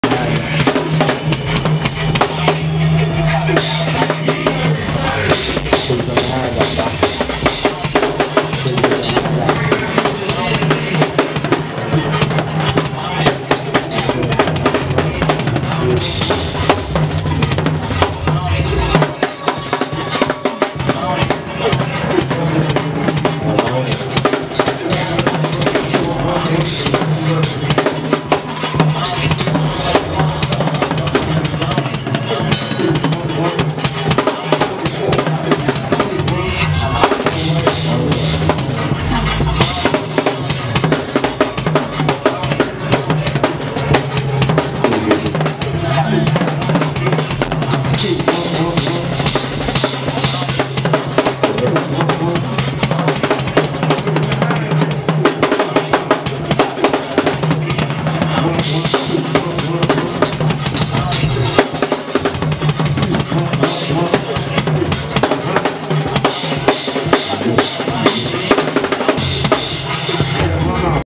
Mark Block on Drums
Just before a show @Club CFE wit TCB BAND